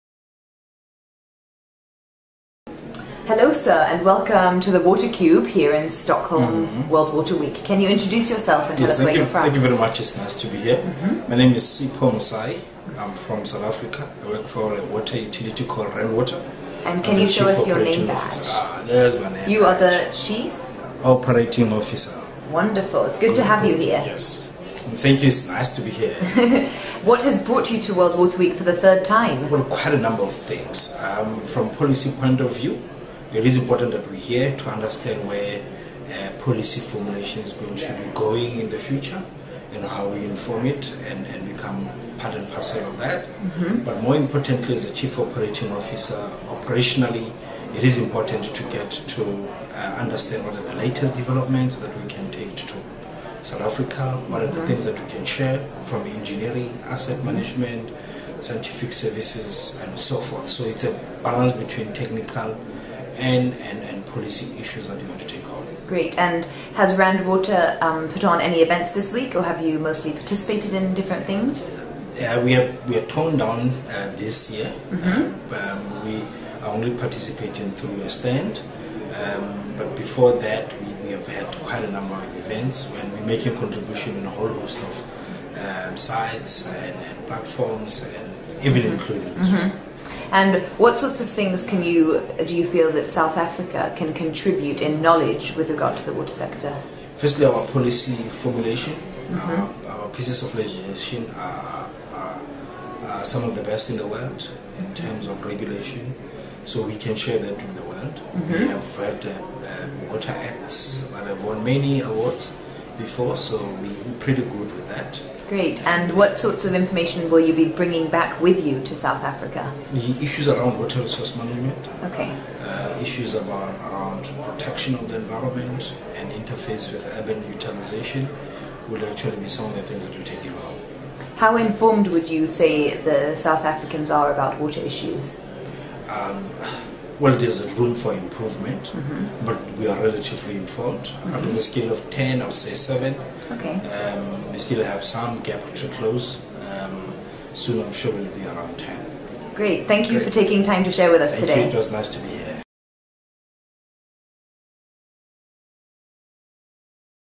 Mr. Sipho Mosai, Rand Water: Learning from South Africa's effective policy formulation.
The chief operating officer of Rand Water, Mr. Sipho Mosai, speak with Helen Downey in the WaterCube at Stockholm World Water Week 2011 about the progressive water work being accomplished in South Africa. Mr. Mosai is happy to be here to gain a greater understanding about future water policy formulations as well as new technical developments in the water sector. (Aug 25, 2011)